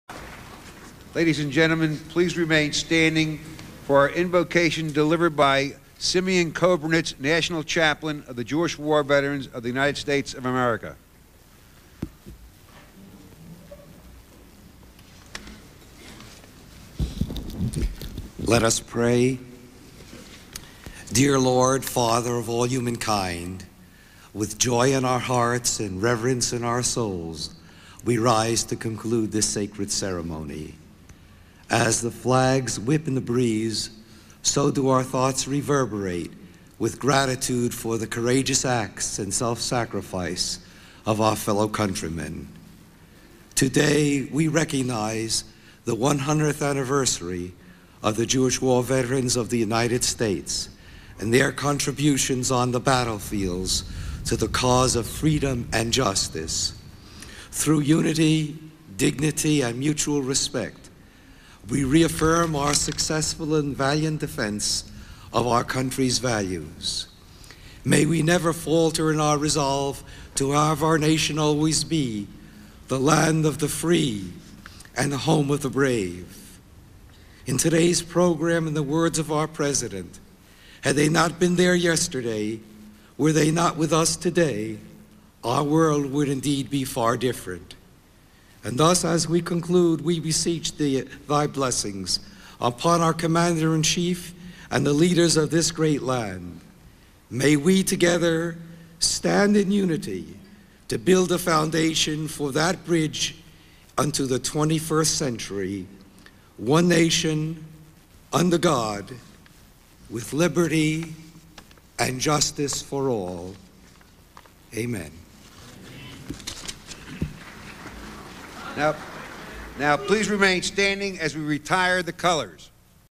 Rabbi Simeon Kobrinetz of Jewish War Veterans: Invocation 11/11/1996 Veterans Day Arlington Cemetery
From November 11, 1996, Veterans Day Memorial Service at Arlington National Cemetery.

Rabbi Simeon Kobrinetz was Senate guest chaplain Feb. 26, 1975.

At the time of his 2011, death, Kobrinetz was president of the Jewish War Veterans.

In his 1996 prayer, Kobrinetz noted it was 100th anniversary of Jewish War Veterans.

WashPost obit 10/18/2011: Simeon Kobrinetz, 83, a rabbi and former Air Force chaplain who later became director of the Chaplain Service of the Department of Veterans Affairs, died Sept. 6 at Walter Reed National Military Medical Center in Bethesda. ... Rabbi Kobrinetz was also instrumental in the construction of a memorial to Jewish chaplains at Arlington National Cemetery.
 After beginning as an active-duty Air Force chaplain in the 1950s, Rabbi Kobrinetz served congregations in Jericho, N.Y., and Philadelphia from 1957 to 1963. He later was director of Hillel campus Jewish groups at the University of Florida and the University of Miami.

In 1969, Rabbi Kobrinetz came to the Washington area to work for what is now the Department of Veterans Affairs. He was director of the VA’s Chaplain Service from 1984 to 1988 and helped reestablish the National Chaplain School in Hampton, Va.

He delivered invocations before presidents and congressional leaders and at national political conventions. He received several distinguished-service awards from the VA before his retirement in 2001. Rabbi Kobrinetz, who served in the Air Force Reserve until 1989, was the first Air Force rabbi to reach the rank of brigadier general.